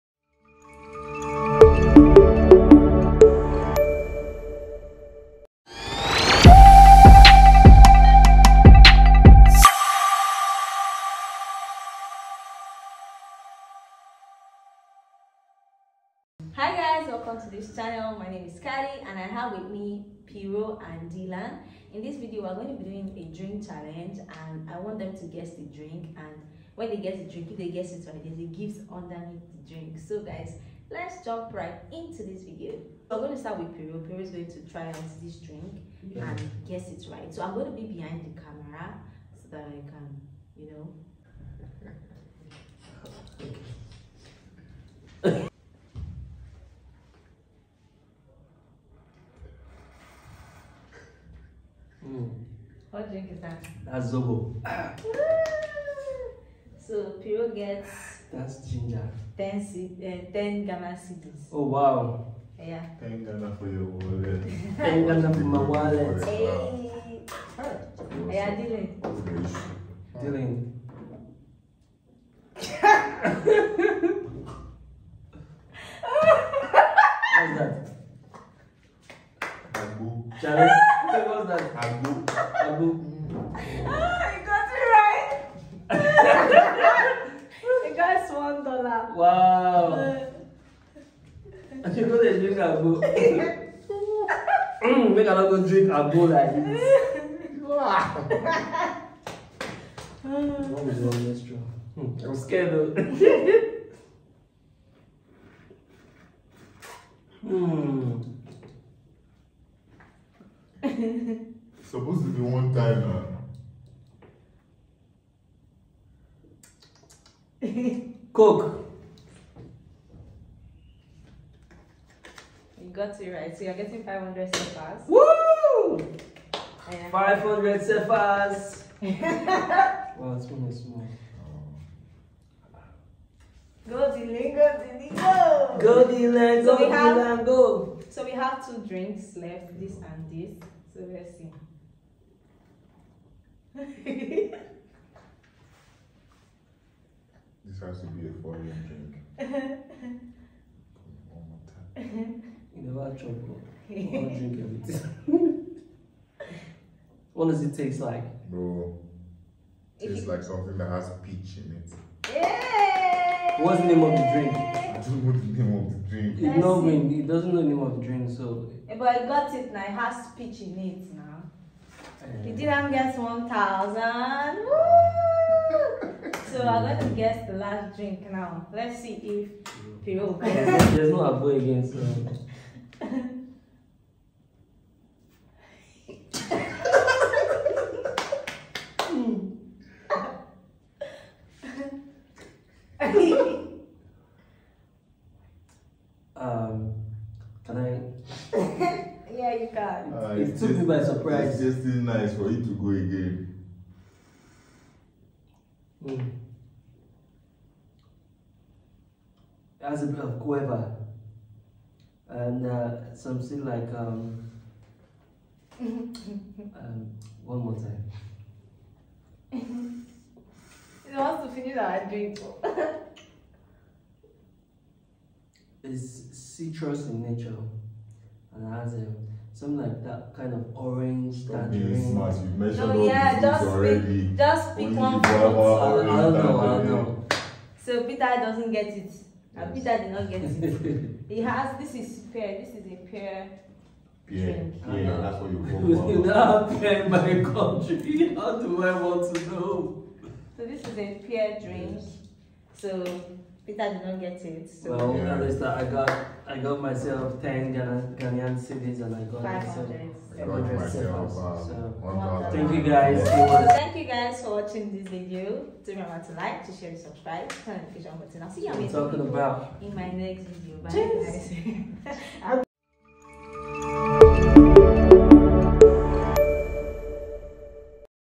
[KADI O] GUESS THE DRINK CHALLENGE KOREAN AND NIGERIAN DRINK EDITION
This video is a guess the drink challenge using Korean and Nigerian local Drink

The more reactions we get, The more events we can offer in Nigeria